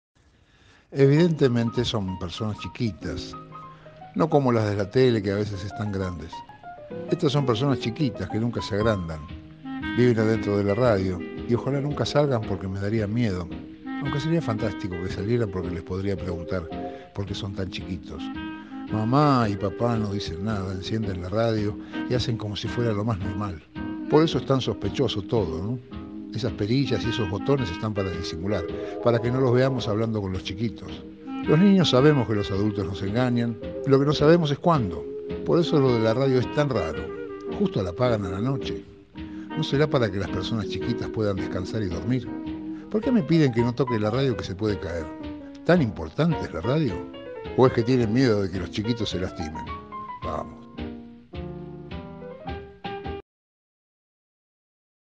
100 años de la radio | Postales: Enanitos
En agosto de 2020, el Centro Cultural Kirchner y el Archivo General de la Nación realizaron un ciclo especial de actividades curado por Carlos Barragán y Pedro Saborido, con motivo del centésimo aniversario de la primera transmisión de radio en el país.

“Postales” es una serie de videos que narra la historia de estos cien años de radio. A través de 10 clips, la serie vincula al medio con la vida cotidiana y con acontecimientos históricos. “Enanitos” desarrolla la hipótesis de un niño: ¿y si adentro de la radio vive gente pequeña? Todos los indicios que él maneja indican que sí.